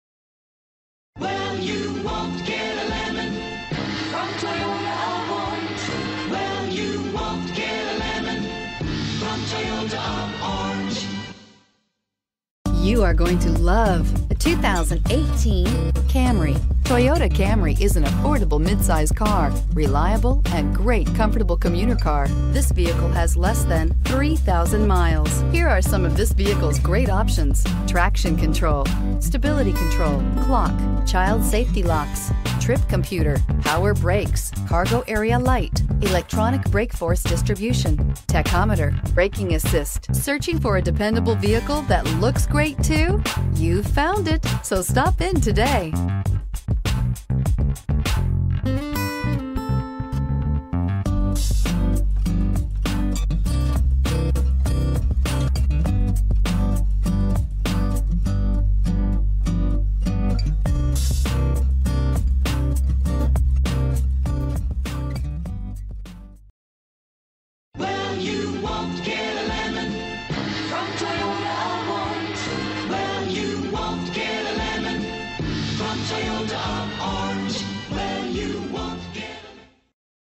2018 Toyota Camry Orange CA B10138
2018 Toyota Camry

1400 North Tustin Ave.

Pick up this White 2018 Toyota Camry, available today at Toyota of Orange. This could be the one you've been searching for.